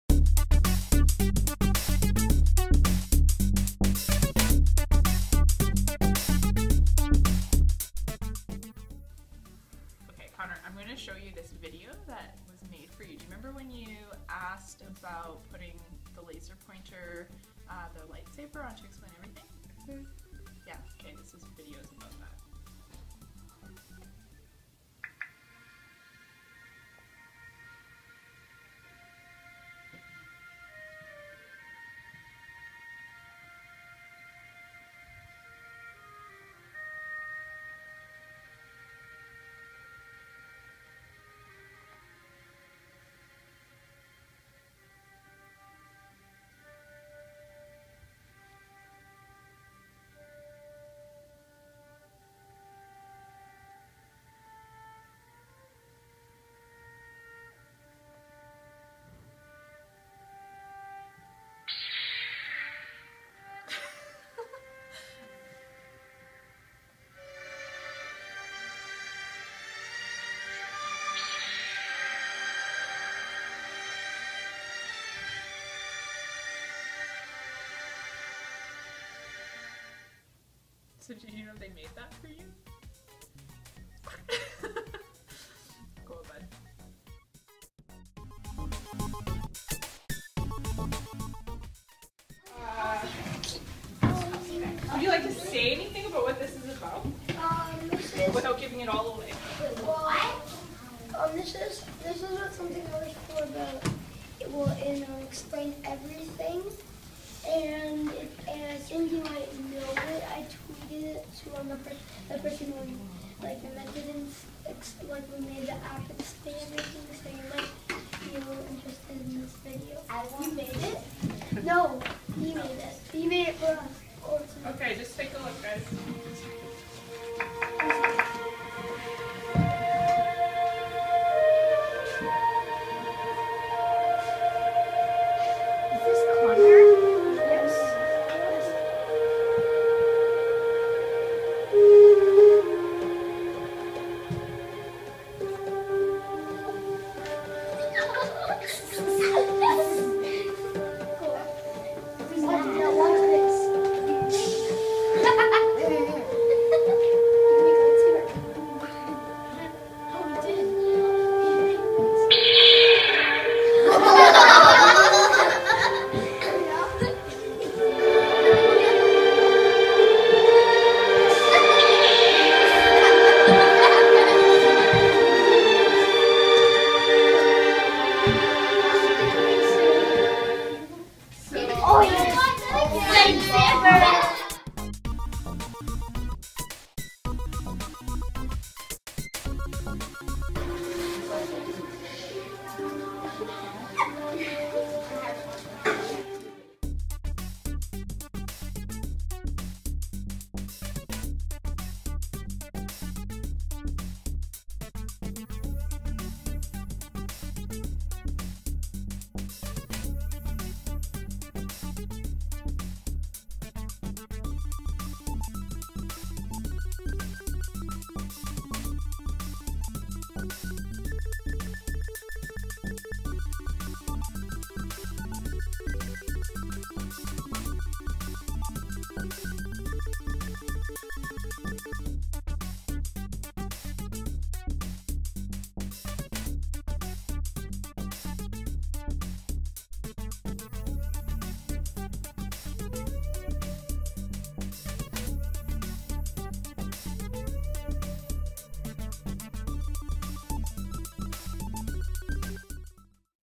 Connor's Idea For Explain Everything
A student in my Grade 3/4 class gets his idea included in the Explain Everything app for iPad. His idea, his reaction when he found out they included it, sharing it with his class, and his reflections.